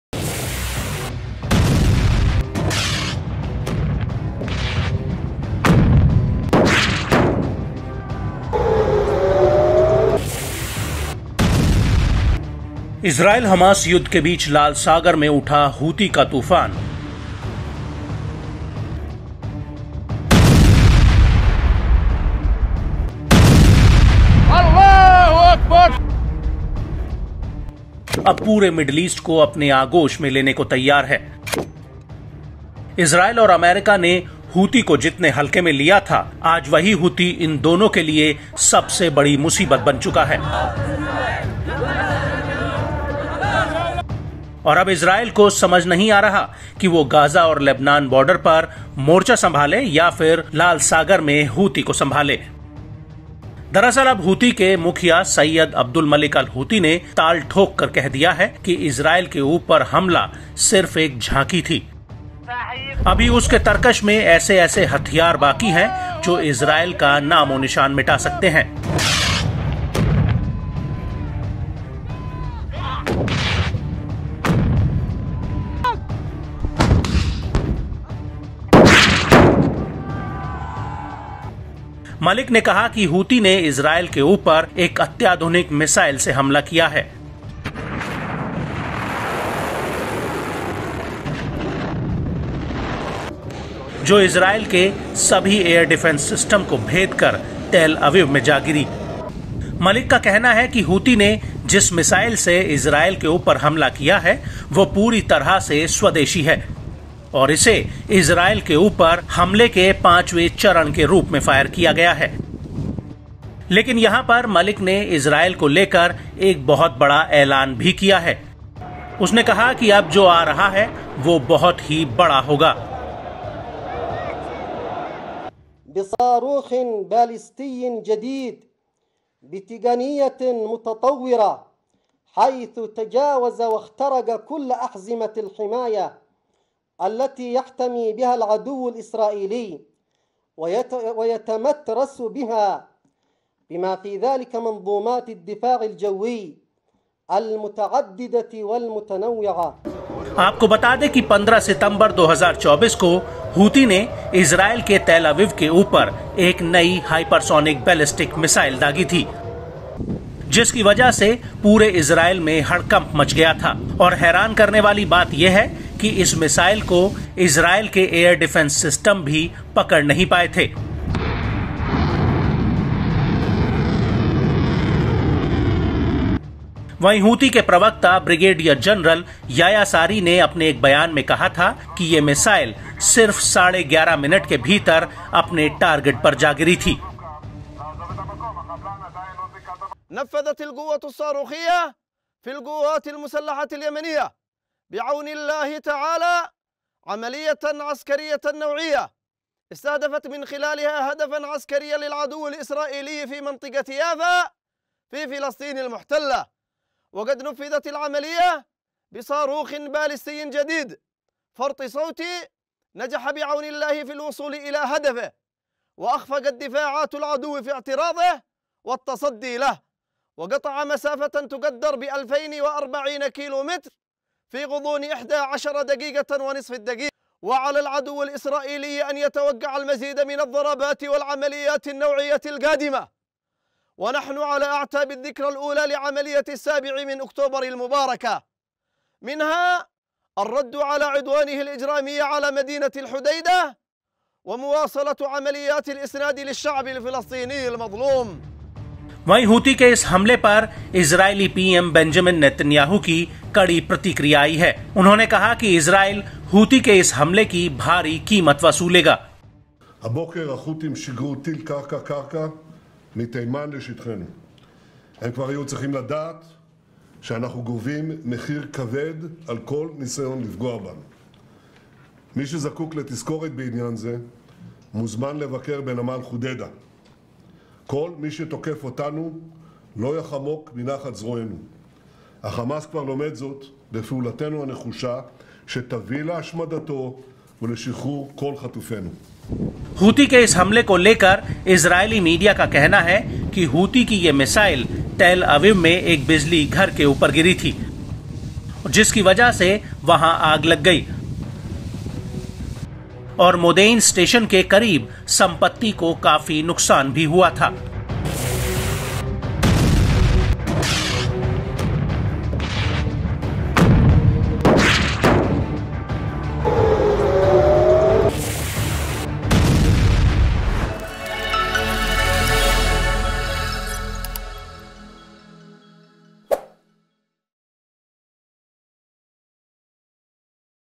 Israel Hamas War: हूती चीफ मिसाइल अटैक के बाद बोला- अब जो होगा वो... | Houthi | N18G
During the Houthi Red Sea Attack amid the Israel Hamas War, Houthi chief Sayyed Abdul Malik-al Houthi has said that the attack on Israel was just a sham. There are still such weapons left in his quiver that can wipe out Israel.

Israel Hamas War के बीच Houthi Red Sea Attack के दौरान Houthi के मुखिया Sayyed Abdul Malik-al Houthi ने ताल ठोककर कह दिया है कि Israel के ऊपर हमला सिर्फ एक झांकी थी. अभी उसके तरकश में ऐसे-ऐसे हथियार बाकी हैं, जो Israel का नामोनिशान मिटा सकते हैं. मलिक ने कहा है कि हूती ने Israel के ऊपर एक अत्याधुनिक मिसाइल से हमला किया है, जो Israel के सभी एयरडिफेंस सिस्टम को भेदकर तेल अवीव में जाकर गिरी.